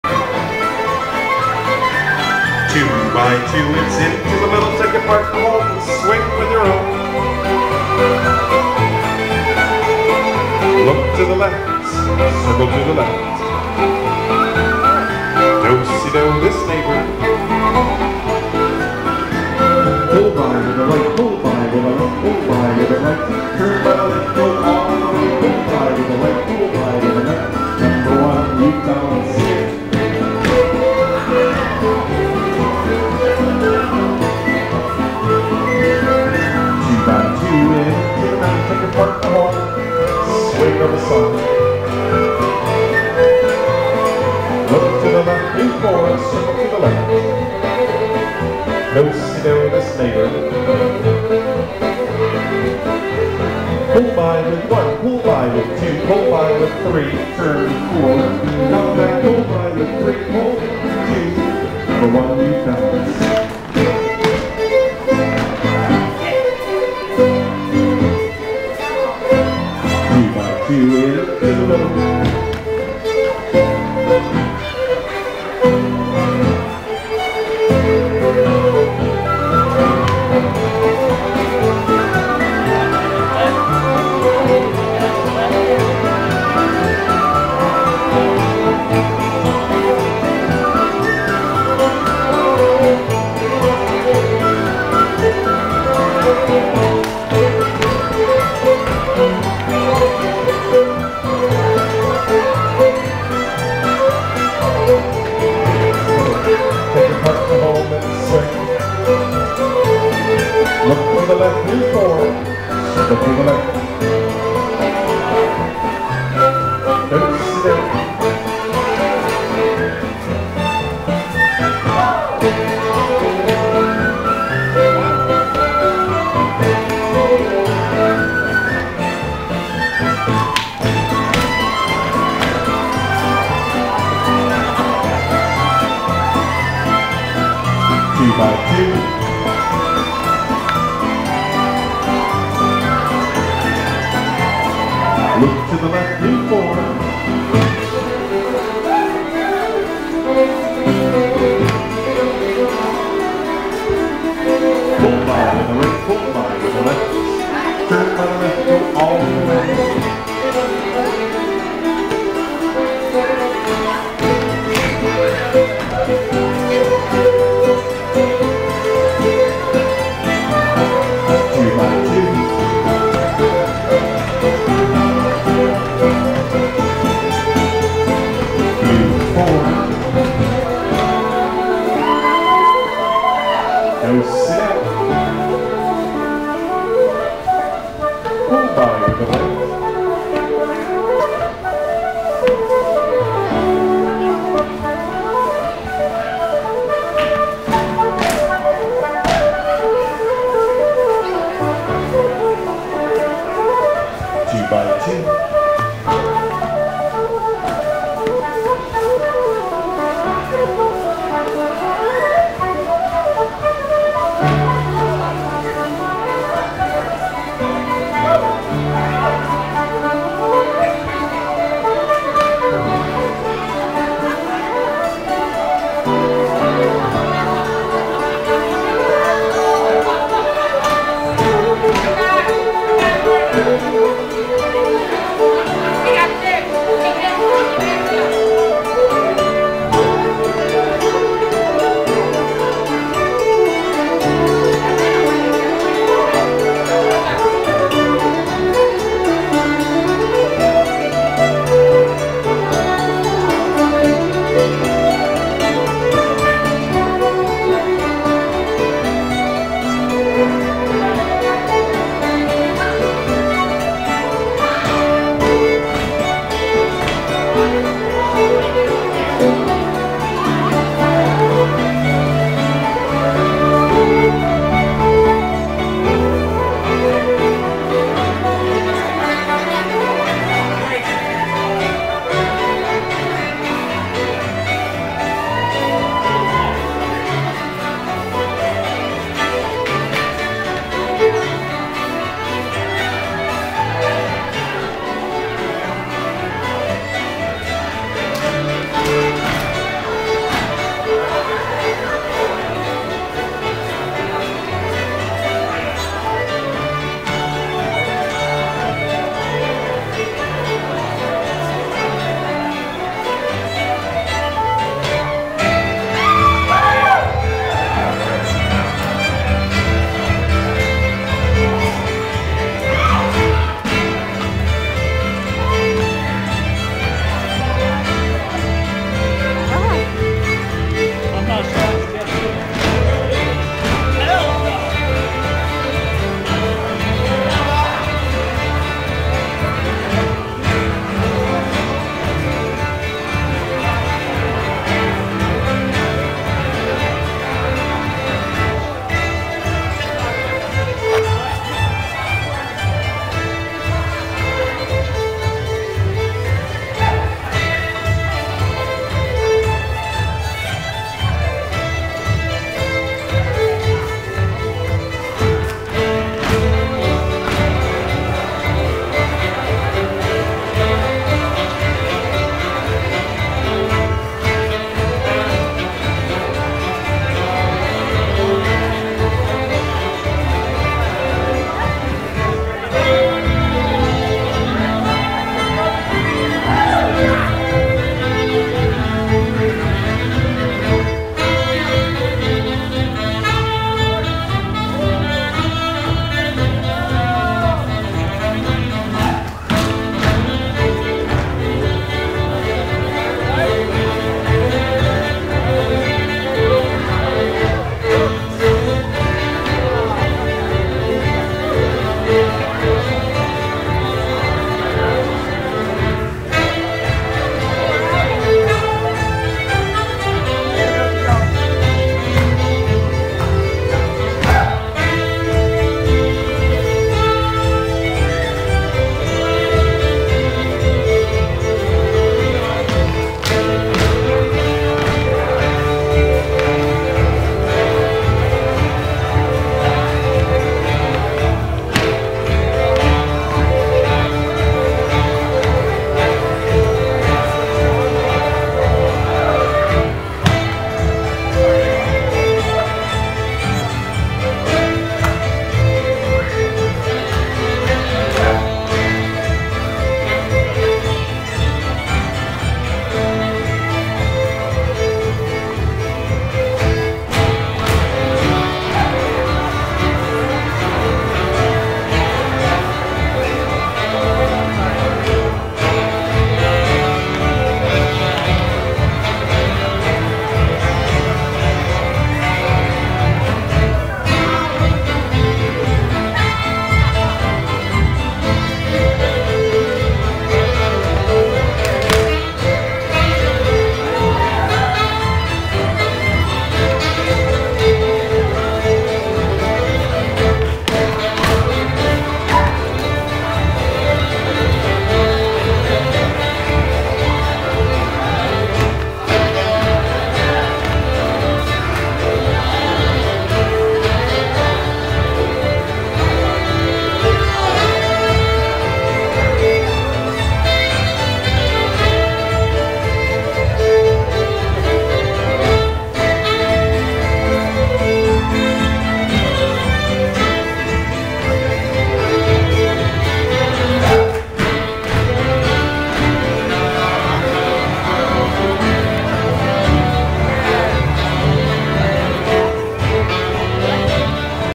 Contra Dance Triangle-Carrboro Swallowtail
what a fun dance!  Great music by Swallowtail. My first contra dance in North Carolina.  Such Great energy!

See if you can find yourself in the video and let me know!!